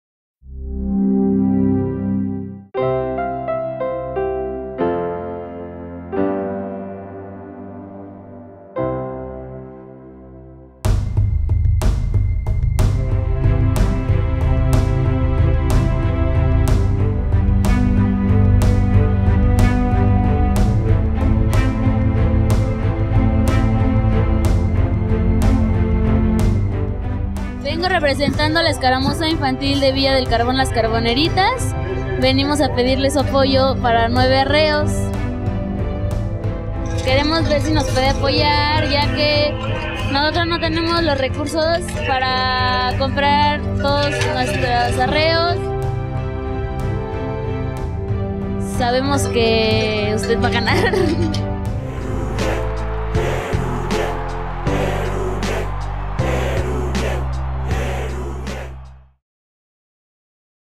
Villa del Carbón - De viva voz
Daleth Mancilla, mujer mexiquense de la escaramuza infantil,   de viva voz solicita el apoyo a Eruviel Ávila, para el arreo.